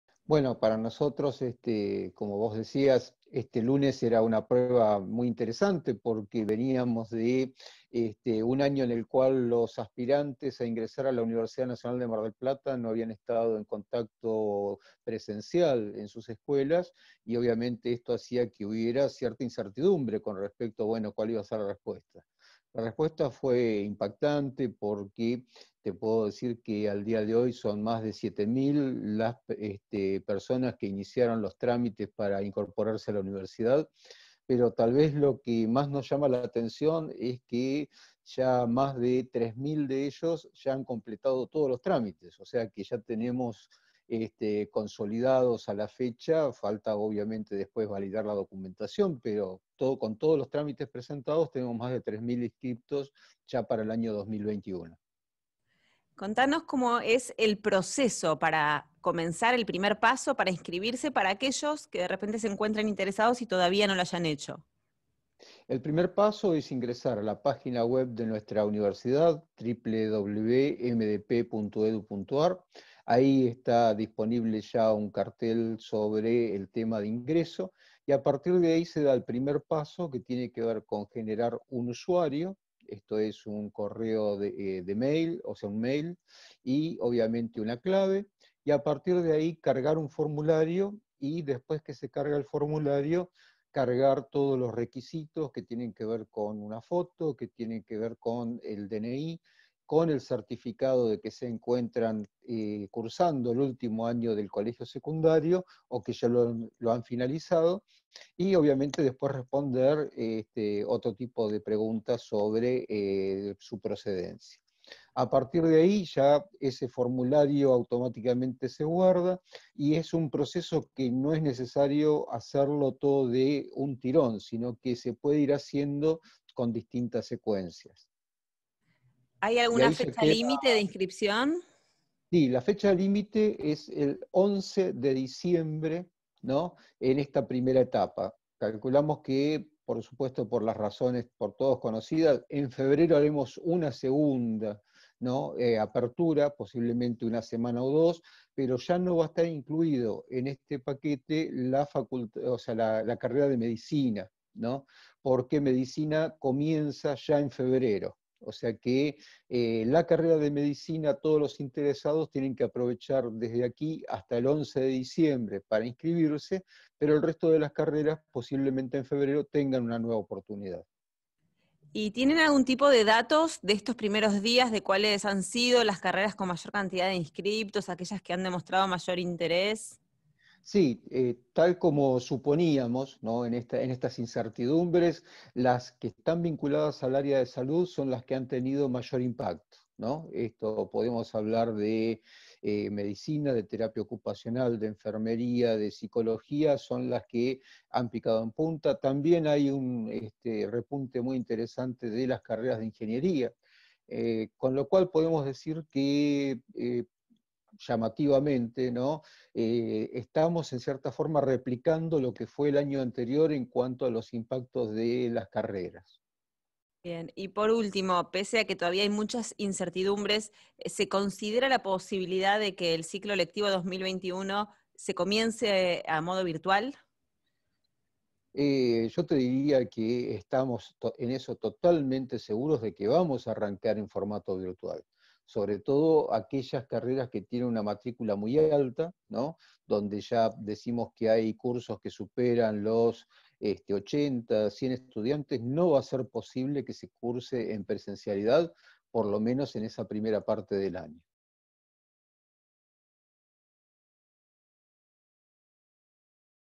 02 10 20 INSCRIPCION EN LA UNIVERSIDAD
ESTA SEMANA COMENZO LA INSCRIPCION A LAS CARRERAS DE LA UNIVERSIDAD NACIONAL PARA EL CICLO LECTIVO 2021. HUBO UNA MAYOR INSCRIPCION A CARRERAS RELACIONADAS CON LA SALUD Y TAMBIEN A INGENIERIA.